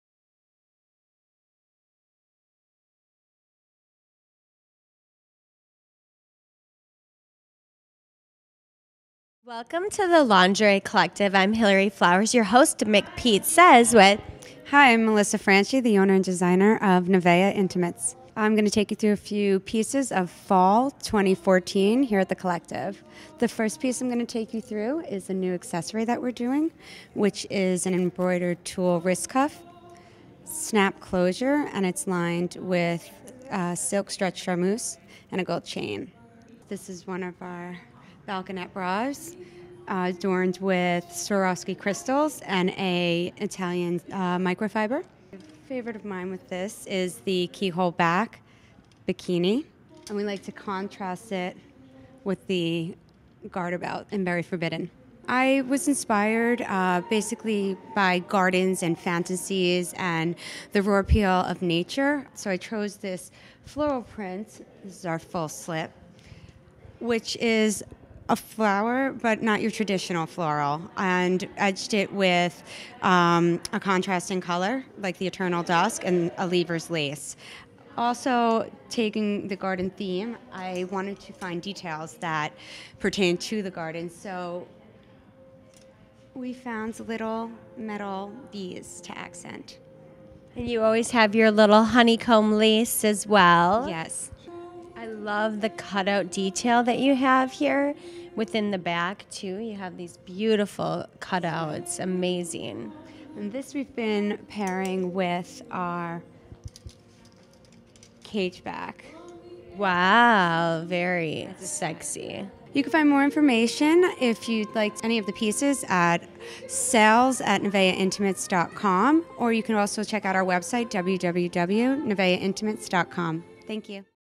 Nevaeh at The Lingerie Collective February 2014
Hillary Flowers interviews Melissa Franchi of Nevaeh Intimates at The Lingerie Collective.